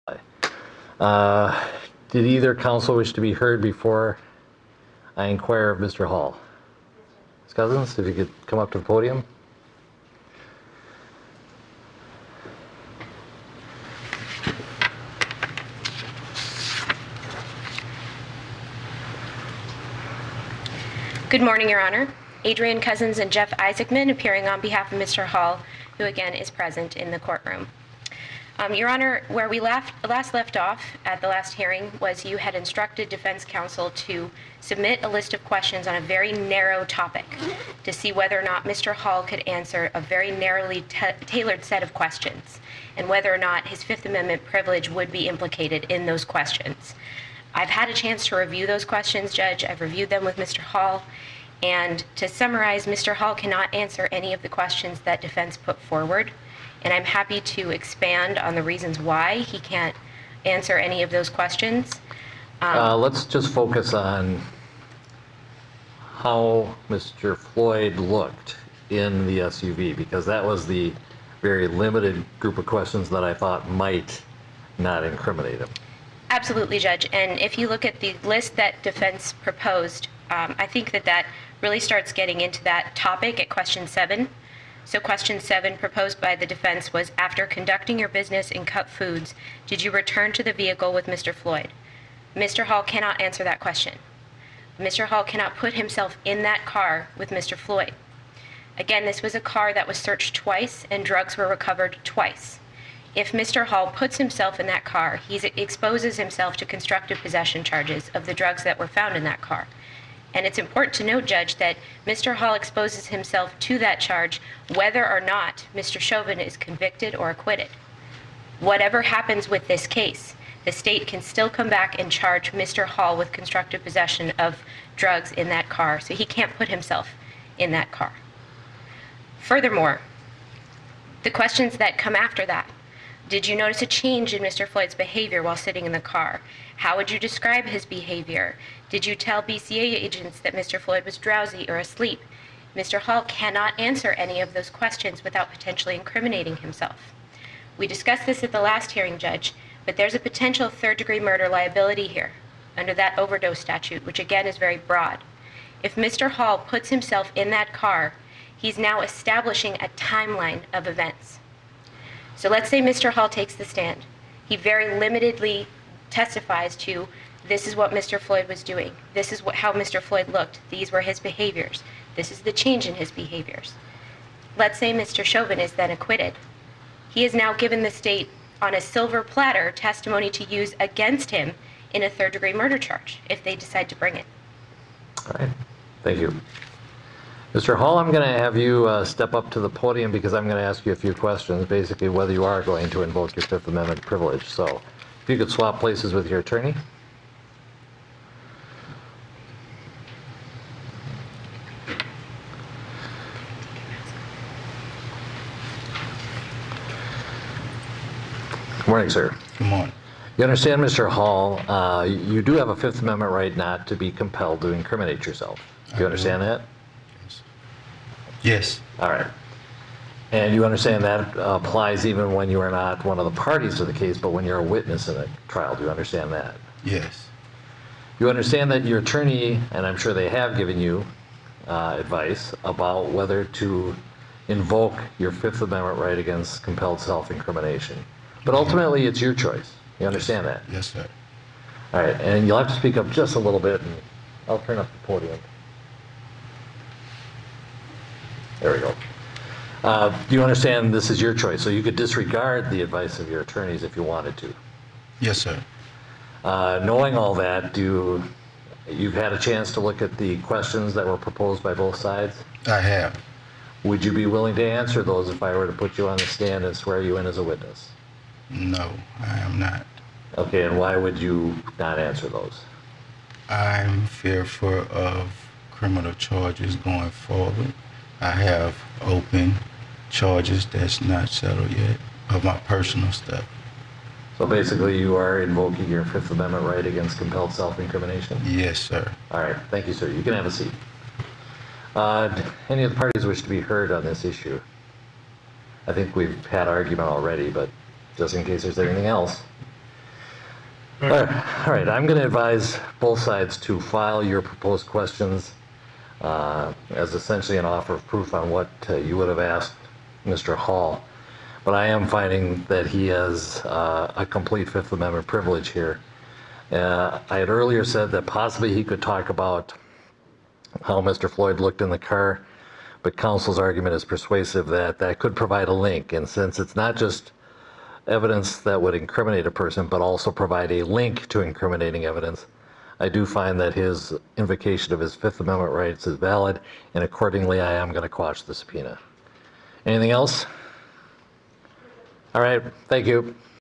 George Floyd's Friend Pleads the Fifth in MN v. Derek Chauvin | COURT TV
WATCH: Judge quashes the subpoena for MorriesHall.

He won't answer any questions as requested by DerekChauvin’s defense after Hall told Judge Cahill he would plead the fifth on the stand.